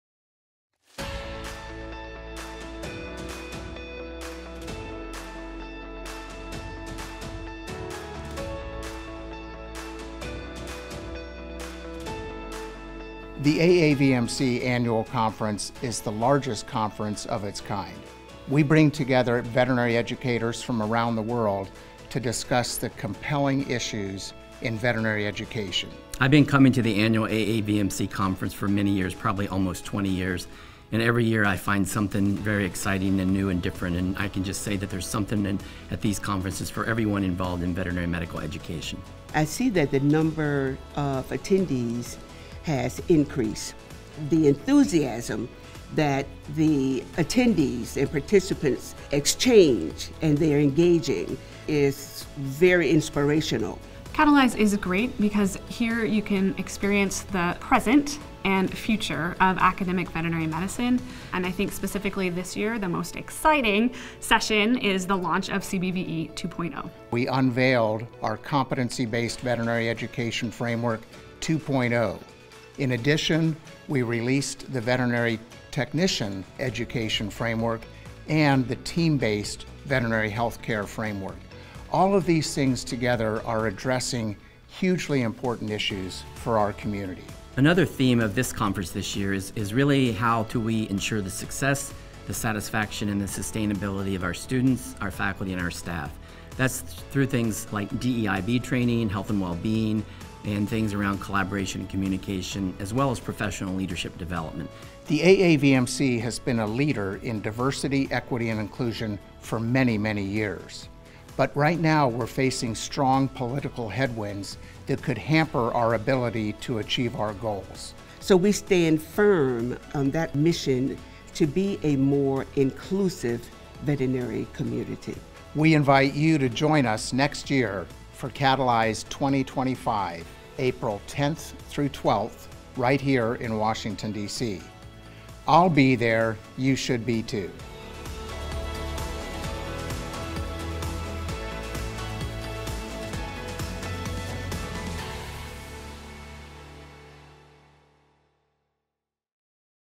AAVMC Catalyze 2024 Recap
Thank you to our attendees, sponsors, and staff who joined us for Catalyze 2024! Check out this video recap of the conference.

Couldn't make it this year? Save the date for Catalyze 2025: April 10-12, 2025 in Washington, D.C.

**The Competency-Based Veterinary Technician Education framework and the Team-Based Veterinary Healthcare competencies were introduced at the 2024 AAVMC Catalyze Conference. The public release of these materials, which are currently being revised and reviewed, is forthcoming.